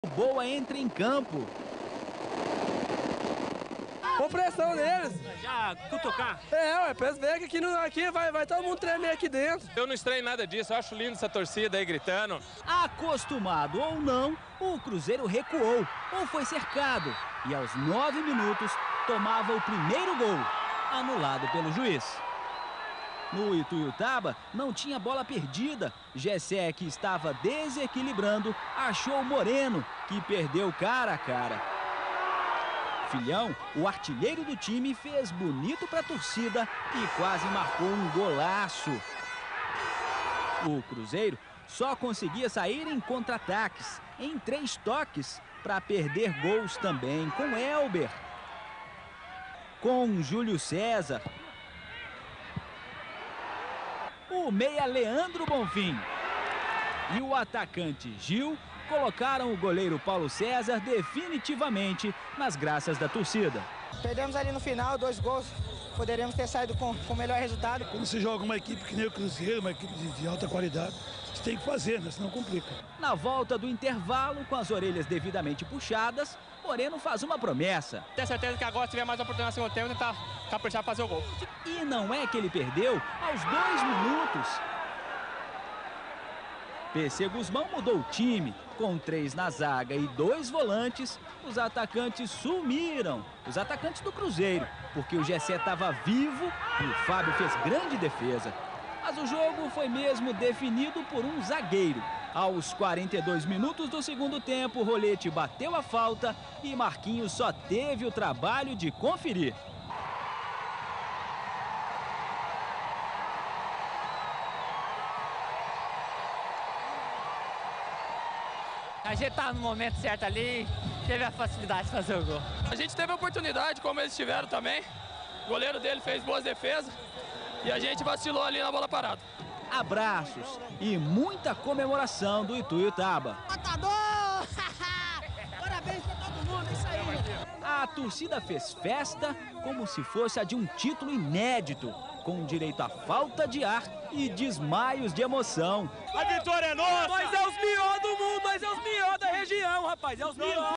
Ituiutaba 1 x 0 Cruzeiro - Campeonato Mineiro 2006
Estádio: da Fazendinha - Ituiutaba-MG
Gol:
1º Marquinho 87'/2 (Ituiutaba)